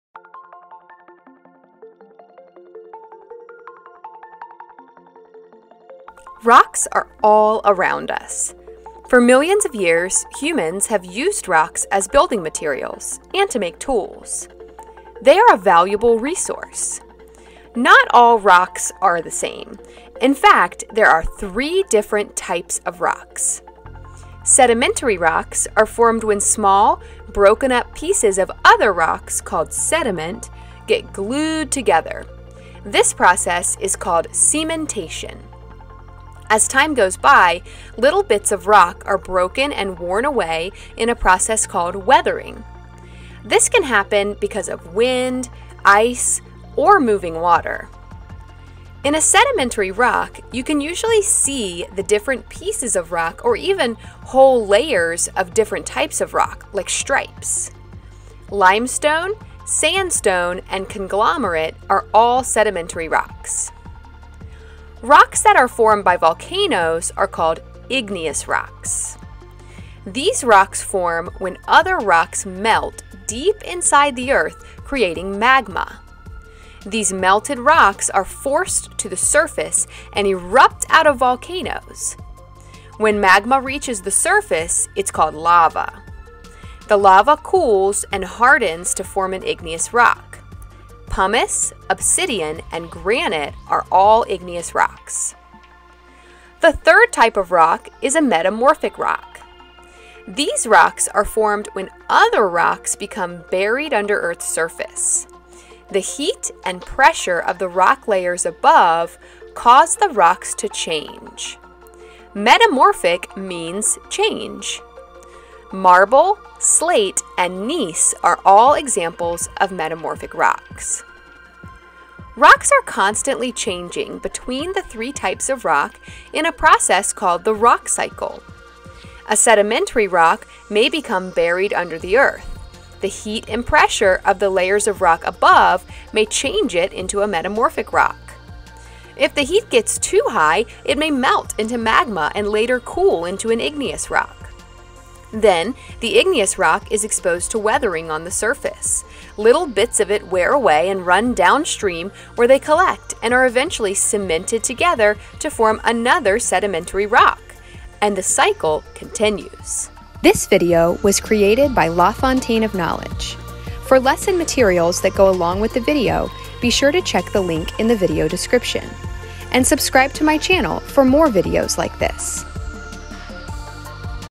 What Is the Rock Cycle?
This video explains how the rock cycle works. It includes examples of sedimentary, igneous, and metamorphic rocks and explains of how they change from one type of rock to another. Click the link for a reading passage and comprehension questions that go along with the video